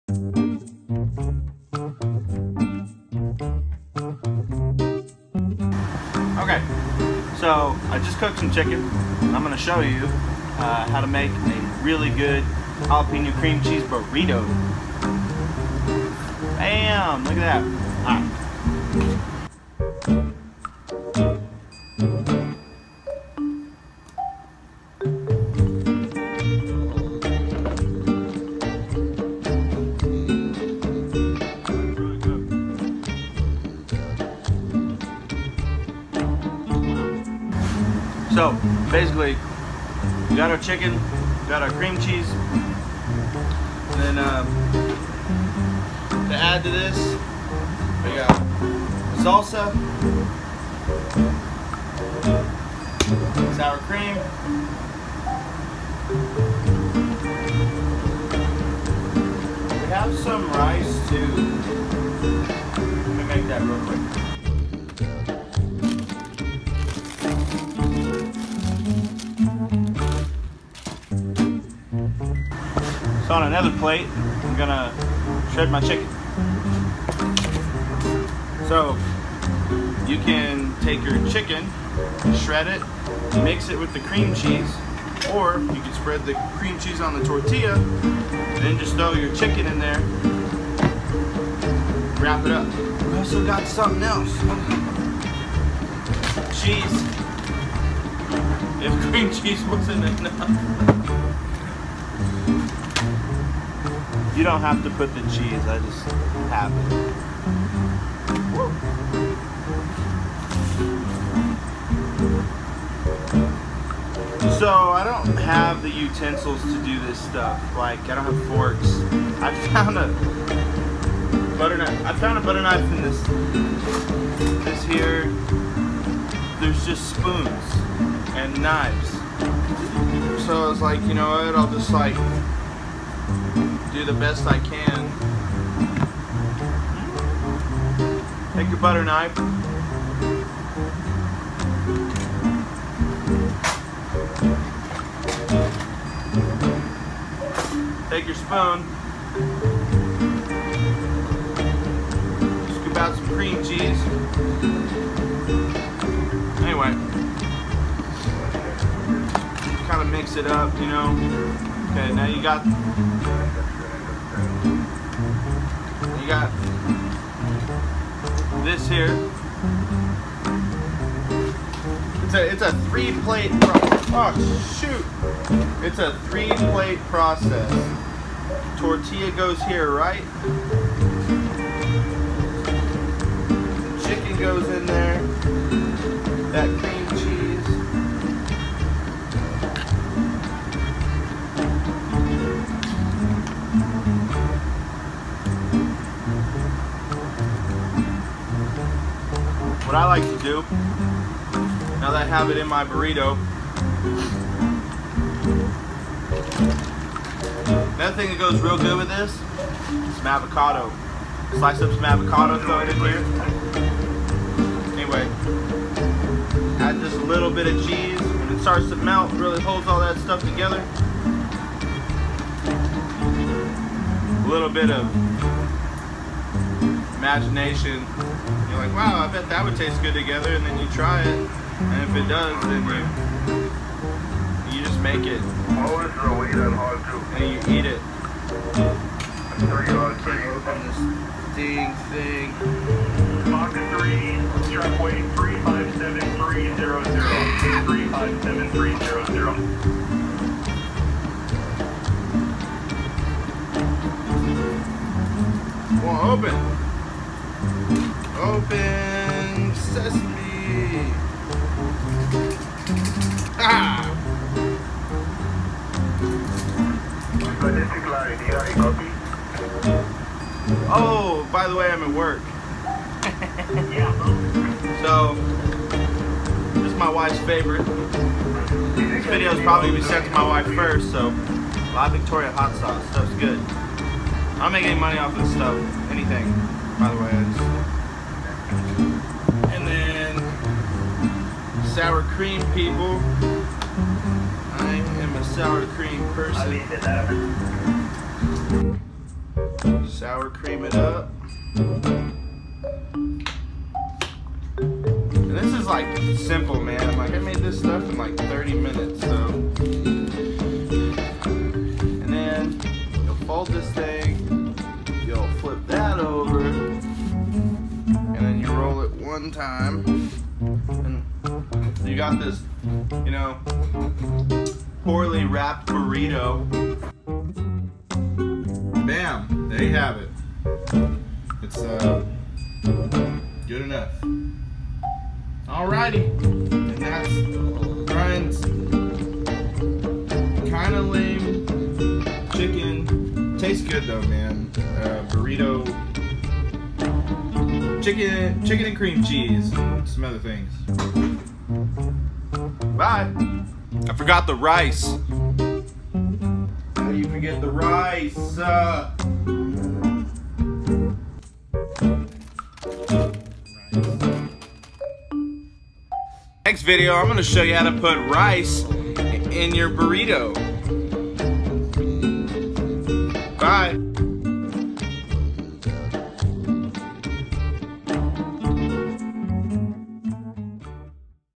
Easy Chicken Burrito!!!
-Barely Edible with Ryan Harding-

Basically it’s a pretty simple burrito...

Cook chicken to desired taste - shred your chicken - combine your chicken with Philadelphia Jalapeño cream cheese (use as much as you’d like here) place a liberal amount in the tortilla - here you can add all the condiments you’d want (salsa, avocado, beans, rice, cheese, pico, sour cream, cilantro) the list is endless here. Then take your burrito and roll it up tight (not like how I rolled mine) place back into your hot skillet with a little oil and brown both sides (it melts all the cheese and makes it amazing) done!!